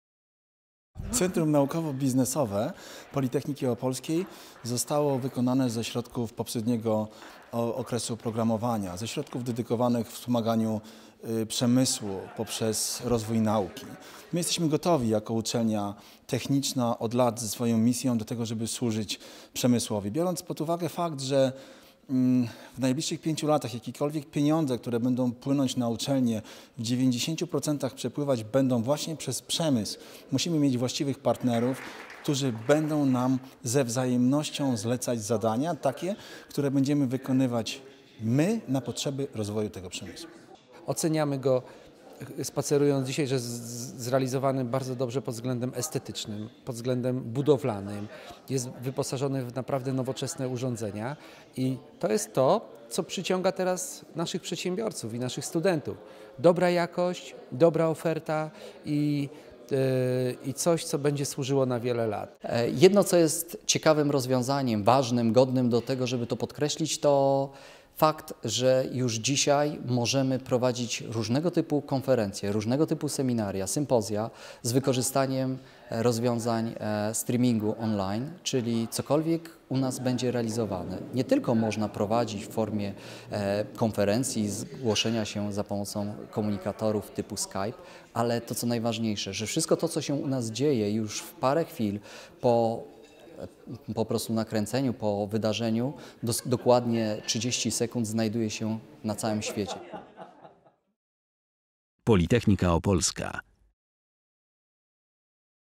Politechnika Opolska uruchomiła Centrum Nauka-Biznes
Innowacyjni przedsiębiorcy i przedsiębiorczy naukowcy od dziś mogą spotykać się pod nowym, ważnym adresem. W zmodernizowanym budynku przy ul. Waryńskiego Politechnika Opolska uruchomiła Centrum Nauka-Biznes, miejsce tworzenia relacji między przemysłem a nauką, które – jak zauważył rektor prof. Marek Tukiendorf - są sobie w regionie wzajemnie potrzebne, zwłaszcza w nowej perspektywie unijnego finansowania.